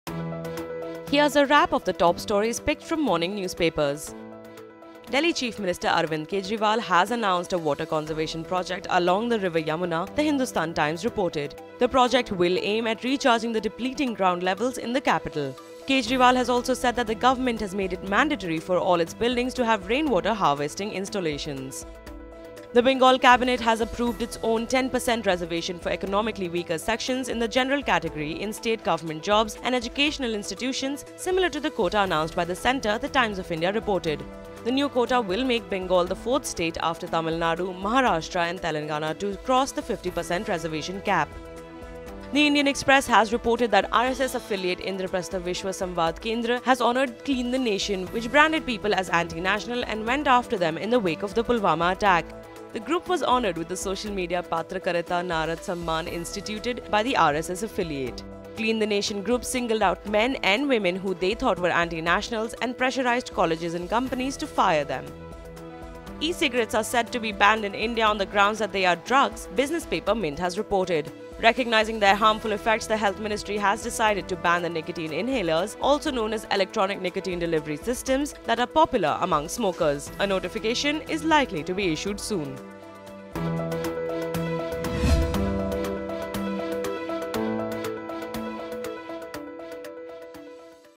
Top headlines picked from major newspapers for you
Here's a wrap of the top stories picked from morning newpapers

Delhi chief minister Arvind Kejriwal has announced a water conservation project along the river Yamuna, the Hindustan Times reported. The project will aim at recharging the depleting ground levels in the Capital. Kejriwal also said the government has made it mandatory for all its buildings to have rainwater harvesting installations

The Bengal cabinet has approved its own 10% reservation for economically weaker sections in the general category in state government jobs and educational institutions, similar to the quota announced by the Centre, the Times of India reported. The new quota will make Bengal the fourth state after Tamil Nadu, Maharashtra and Telangana to cross the 50% reservation cap.